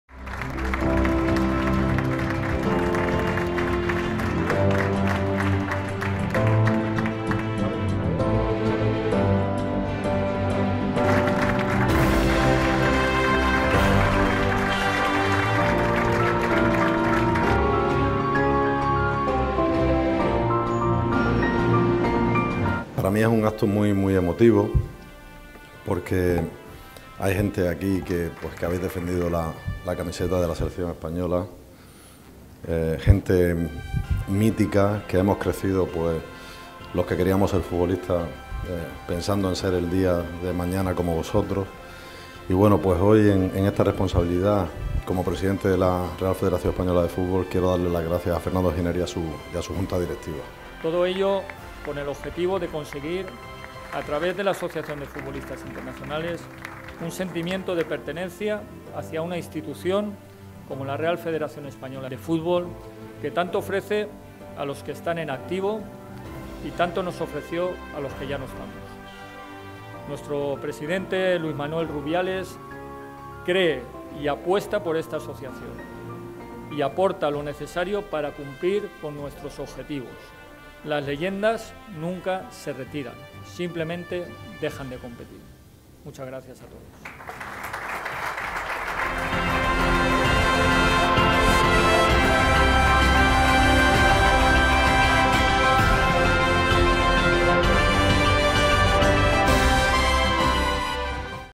Luis Rubiales, con Leyendas España en Barcelona | 🔴 SEFUTBOL
La ciudad de Barcelona ha sido la tercera parada de los 'Encuentros de Leyendas' que se están llevando a cabo por toda la geografía española, una cita en la que florecen infinidad de recuerdos y anécdotas entorno a una pasión, el fútbol, y el orgullo de haber vestido la camiseta de la Selección a lo largo de su historia porque la Roja une a todos.
Futbolistas de varias generaciones de la Roja nacidos en Cataluña o que han desarrollado gran parte de su trayectoria profesional en sus clubes recibieron su 'Dorsal de Leyenda' de manos del presidente de Asociación Española de Futbolistas Internacionales (AEdFI), Fernando Giner, y del presidente de la RFEF, Luis Rubiales.
Desde futbolistas como Sadurní, que debutó con la selección en 1963, hasta otros en activo como Marc Bartra (Real Betis) y una representación de la selección de Luis Enrique formada por Jordi Alba, Ferran Torres, Pedri González y Eric García, pasando por Capdevila, Quique Costas, Rexach, Pichi Alonso, Víctor Muñoz, Albert Celades, Albert Luque, Sergio Garcia o Ignacio Camacho, entre otros, recibieron un sentido homenaje y reconocimiento que también tuvieron los familiares de Justo Tejada y Martí Vergés.
“Gracias por todo lo que habéis representado durante la historia de la selección, por lo que habéis transmitido, estoy orgulloso de todos vosotros, a algunos os he tenido en cromos, otros habéis sido compañeros de profesión o rivales en el campo, estoy orgulloso de representaros como presidente de AEdFI haciendo una labor que es muy importante para todos vosotros", afirmó Fernando Giner, que explicó todas las acciones deportivas y sociales para obtener los recursos para los compañeros que lo necesitan, antes de concluir con una reflexión que hace propia la asociación: "Las Leyendas nunca se retiran, simplemente dejan de competir".
No era una cita más, la Selección llevaba 18 años sin jugar en Barcelona y, como indicó Luis Rubiales, "no puedo comprender haber estado tanto tiempo sin venir aquí. En todo lo que dependa de nosotros no volverá a ocurrir. Aquí hay gente mítica con la que crecimos aquellos que queríamos ser futbolistas". El Presidente de la RFEF también ha querido agradecer a Fernando Giner y a su Junta Directiva por el cambio que ha habido en la asociación y que "permite estar más cerca de los internacionales. Es un evento muy emotivo, espero que disfrutéis mucho y que la Junta os siga devolviendo todo lo que habéis hecho por nosotros".
La jornada sirvió también para comunicar el acuerdo con el movimiento 'Ponte en Marcha', que recauda fondos para la enfermedad rara de Von Hippel-Lindau (a través de la Alianza Española de familias VHL), una patología que afecta a una de cada 36.000 personas en España. Su directora, Rocío de Zárate, valoró muchísimo "la visibilidad para un movimiento benéfico que acaba de nacer. Vosotros, cuando nacisteis, soñabais con ser jugadores internacionales; nosotros empezamos con una gala benéfica, luego una carrera y ahora hemos dado el salto a Barcelona. Esperemos algún día ser leyendas también".
Antes de finalizar y realizar la tradicional foto de familia, se contó con la presencia de los periodistas Ramón Besa (El País) y Alfredo Martínez (Onda Cero), que valoraron el significado que tienen para la selección los futbolistas catalanes, así como recordar noches "memorables" que la Roja ha vivido en Cataluña, además de resaltar que en la actualidad, con Luis Enrique al mando, "hay una idea de juego".